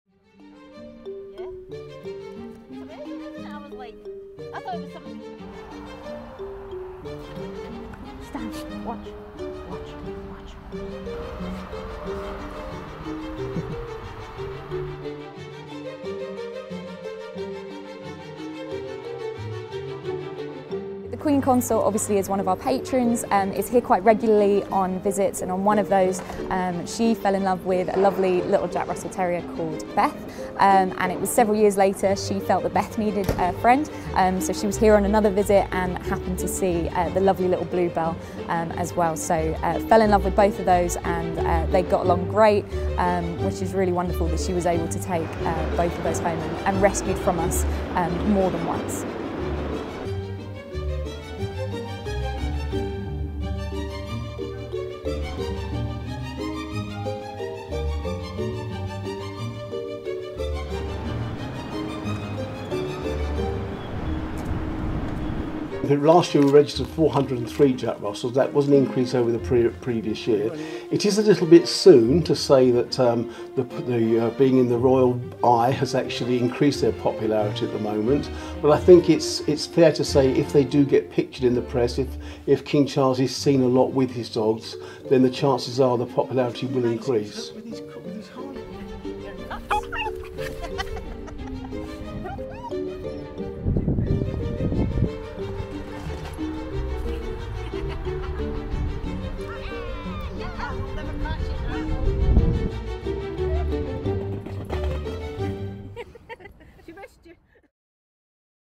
Move over Corgis: Jack Russell becomes new royal top dog | AFP
Beloved by the late Queen Elizabeth II, corgis have become an enduring symbol of the British royal family. But there is a new breed on the Buckingham Palace block -- the plucky Jack Russell Terrier -- with King Charles III and Queen Camilla owning two rescue dogs named Bluebell and Beth.